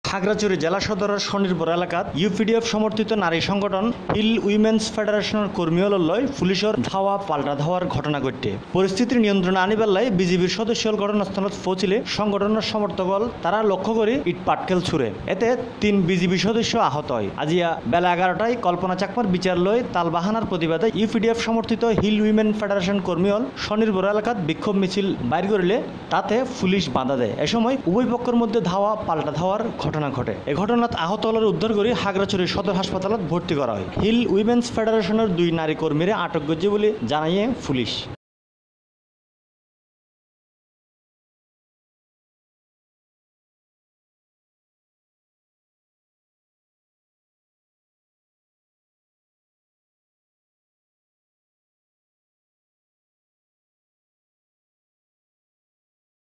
খাগড়াছড়িতে হিল উইমেন্স ফেডারেশনের মিছিলে বাঁধা, তিন বিজিবি সদস্য আহত; আটক ২
খাগড়াছড়ি জেলা সদরের স্বনির্ভর এলাকায় ইউনাইটেড পিপলস ডেমোক্রেটিক ফ্রন্ট (ইউপিডিএফ) সমর্থিত নারী সংগঠন হিল উইমেন্স ফেডারেশনের কর্মীদের সাথে পুলিশের ধাওয়া-পাল্টা ধাওয়ার ঘটনা ঘটেছে।

পরিস্থিতি নিয়ন্ত্রণে আনতে বিজিবি’র সদস্যরা ঘটনাস্থলে পৌঁছালে সংগঠটির সমর্থকরা তাদের লক্ষ্য করে ইট পাটকেল ছুঁড়লে তিন বিজিবি সদস্য আহত হন।

আজ বেলা ১১ টার দিকে ‘কল্পনা চাকমার বিচার’ নিয়ে তালবাহানার প্রতিবাদে ইউপিডিএফ সমর্থিত হিল উইমেন্স ফেডারেশনের কর্মীরা স্বনির্ভর এলাকায় বিক্ষোভ মিছিল বের করলে তাতে পুলিশ এসে বাঁধা দেয়।

এসময় উভয়পক্ষের মধ্যে ধাওয়া পাল্টা ধাওয়ার ঘটনা ঘটে। পরে পরিস্থিতি নিয়ন্ত্রনে আনতে বিজিবি সদস্যরাও ঘটনাস্থলে পৌঁছালে তাদের লক্ষ্য করে ইট পাটকেল ছুঁড়লে তিন বিজিবির সদস্য আহত হন। আহতদের উদ্ধার করে খাগড়াছড়ি সসদর হাসপাতালে ভর্তি করা হয়।

এ ঘটনায় হিল উইমেন্স ফেডারেশেনের ২ নারী কর্মীকে আটক করে পুলিশ।

এদিকে হিল উইমেন্স ফেডারেশনেরও কয়েকজন আহত হয়েছে বলে দাবী করা হয়েছে সংগঠনটির পক্ষ থেকে।

পুলিশের ভারপ্রাপ্ত কর্মকর্তা (ওসি) তারেক মো: আব্দুল হান্নান জানান, অনুমতি ছাড়া বিক্ষোভ করায় তাদের বাঁধা দেয়া হয়েছে।